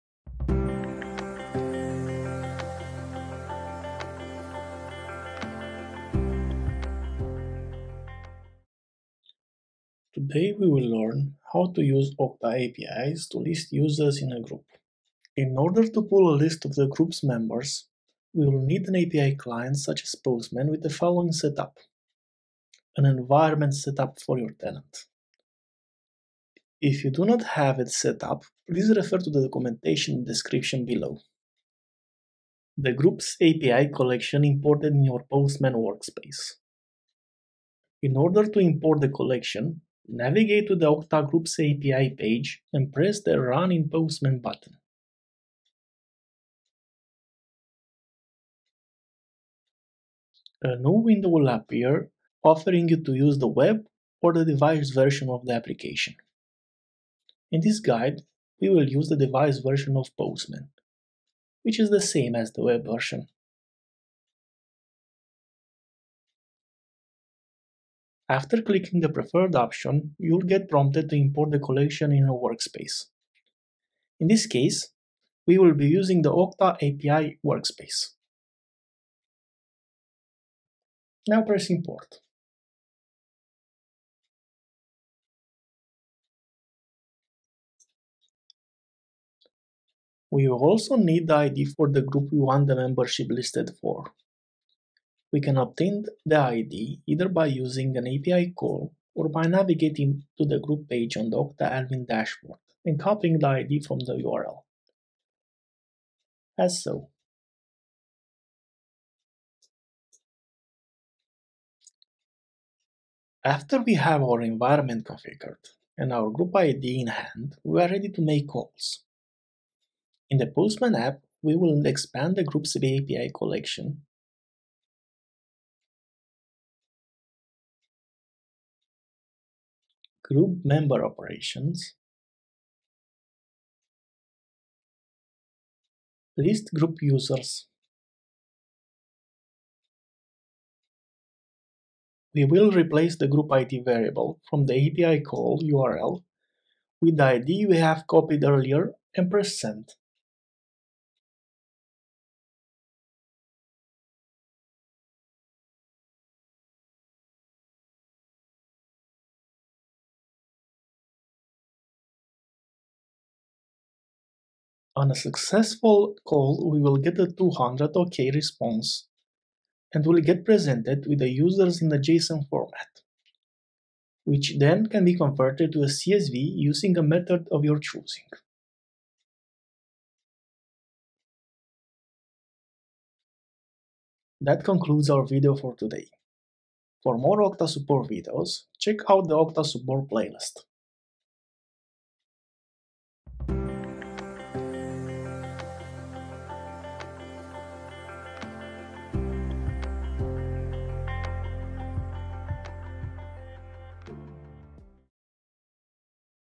Listing Group Users with Okta APIs | Okta Support
Learn how to use Okta APIs to list users in a group.